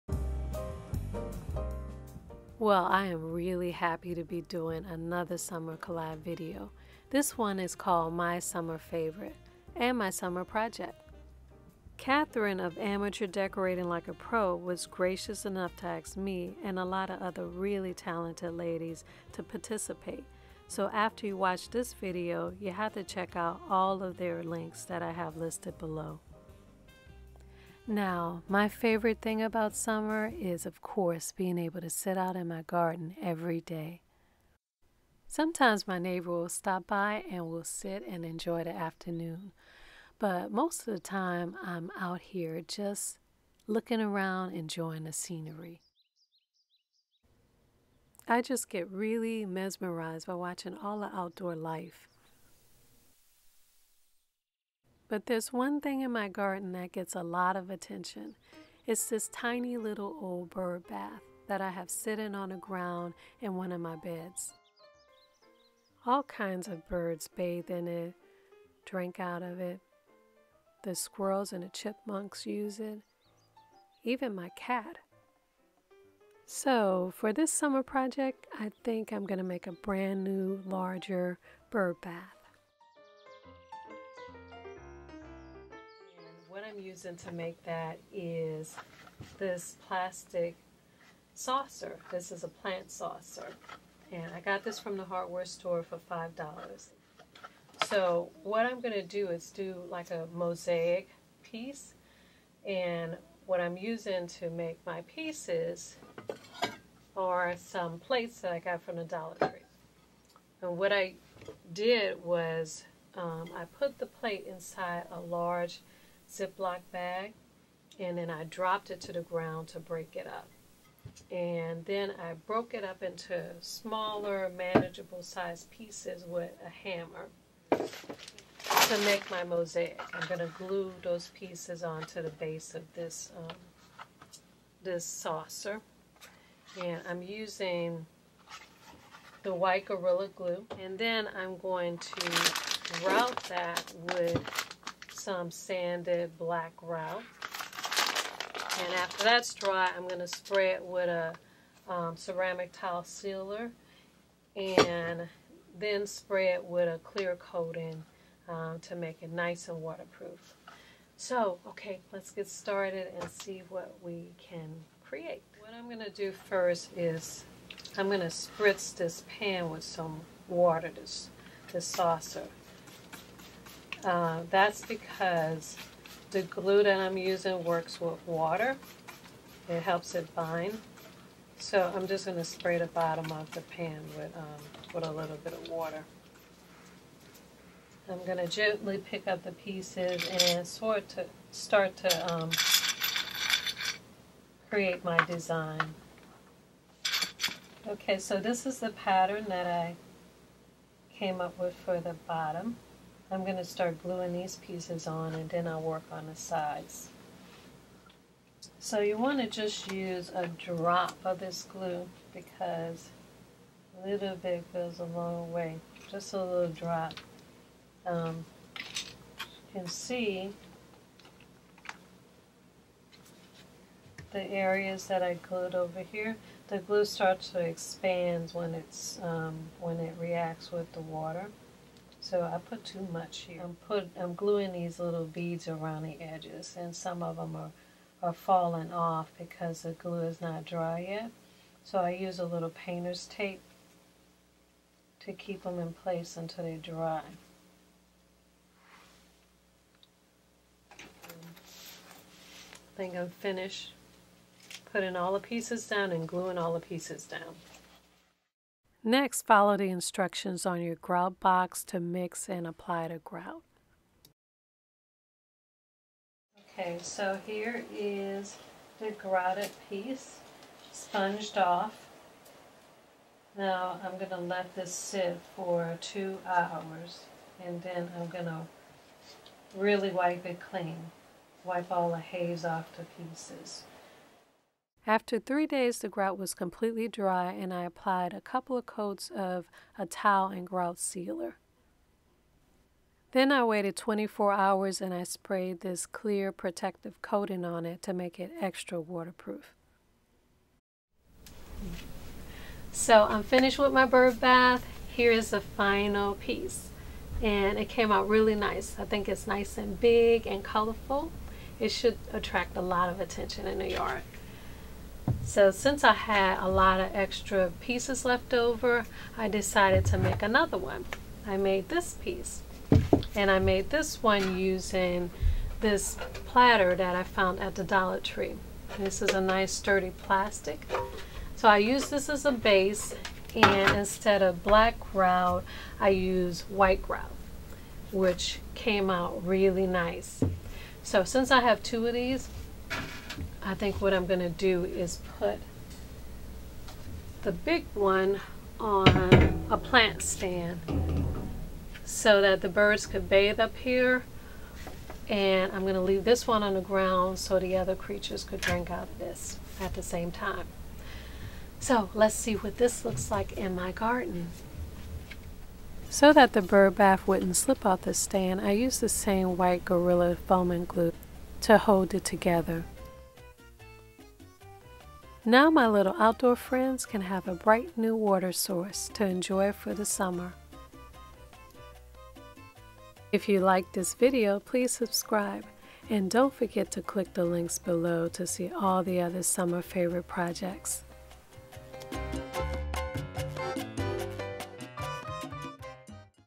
Summer Fav & Project - DIY Bird Bath
Now you can pick up all our products and favorite crafting supplies for easy ordering and quick delivery all in one place!!

Watch how I make my love of bird watching a little easier by creating unique bird baths for my garden.

Toni Scott-Danie:l Producer/Host